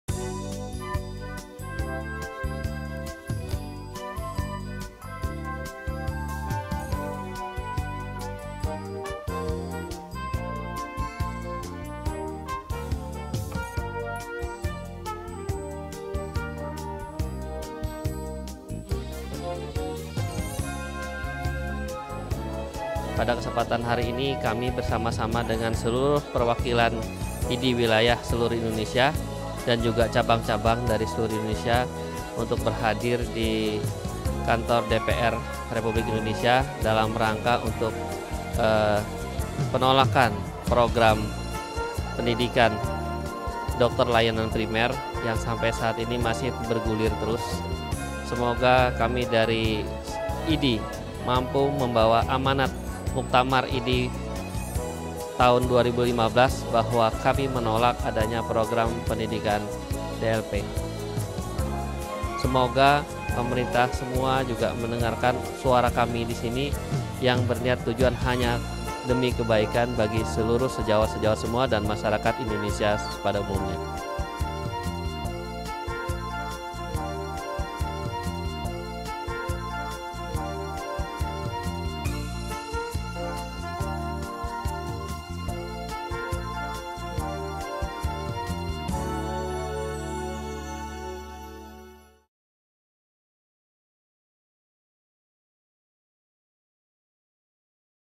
PENOLAKAN PROGRAM PENDIDIKAN DOKTER LAYANAN PRIMER SETARA SPESIALIS OLEH IDI
Pernyataan seluruh perwakilan IDI (Ikatan Dokter Indonesia) salah satunya Wilayah Kalimantan Selatan terhadap Program Pendidikan Dokter Layanan Primer (DLP) yg setara Spesialis, sesuai amanat Muktamar IDI di Medan 2015